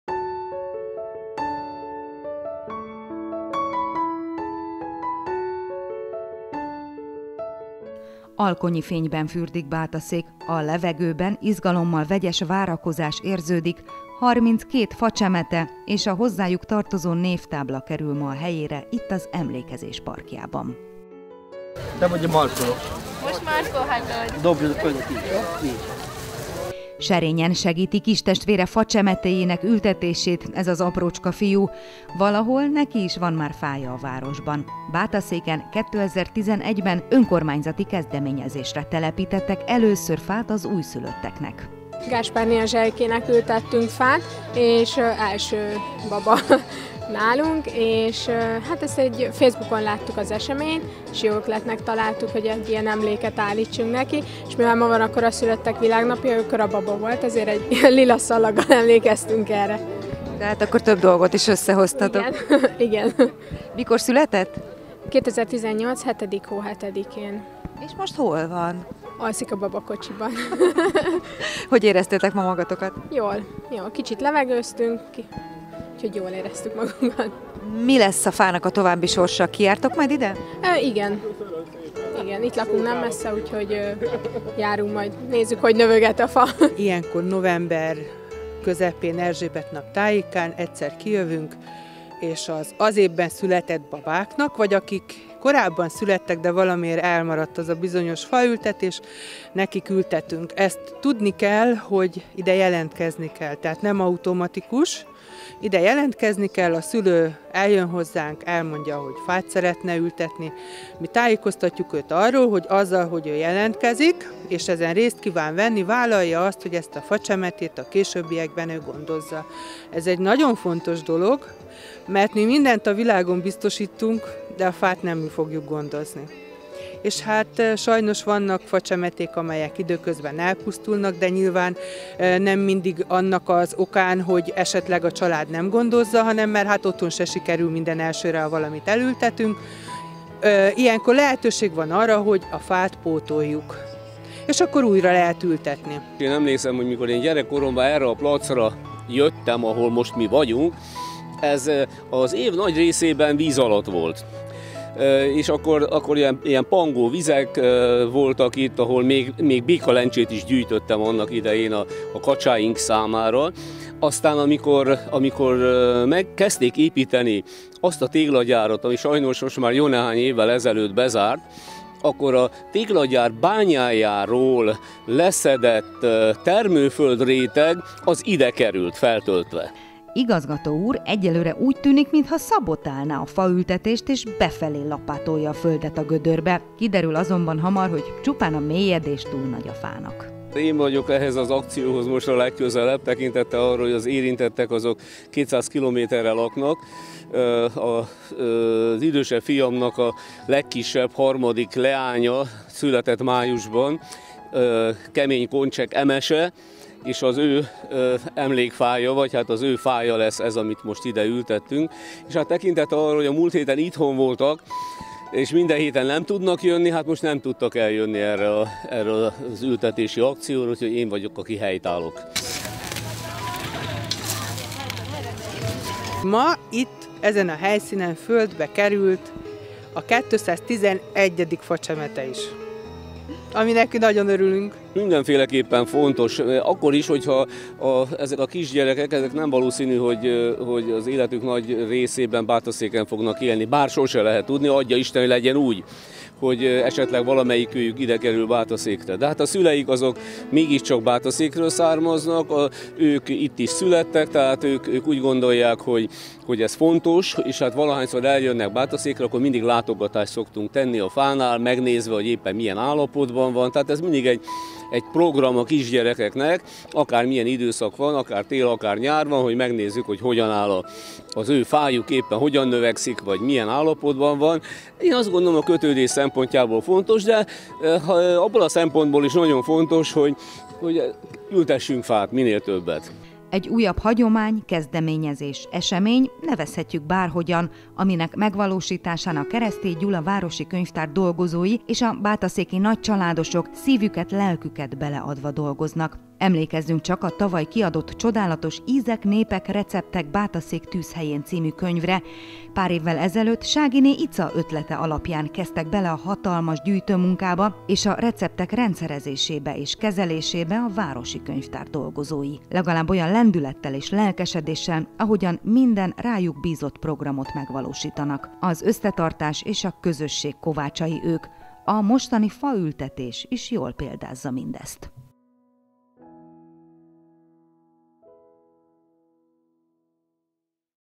Ismét fát ültettek Bátaszéken azoknak a babáknak, akik az elmúlt évben születtek. 20181119
Ismét fát ültettek Bátaszéken azoknak a babáknak, akik az elmúlt évben születtek. Nem csak a város ünnepli ilyenkor az új városlakóit, hanem a családok is. Ilyenkor gyerekek, szülők, nagyszülők, testvérek és barátok közösen ültetik el azt a facsemetét, ami jelképezi újszülött kisbabájuk érkezését a családba.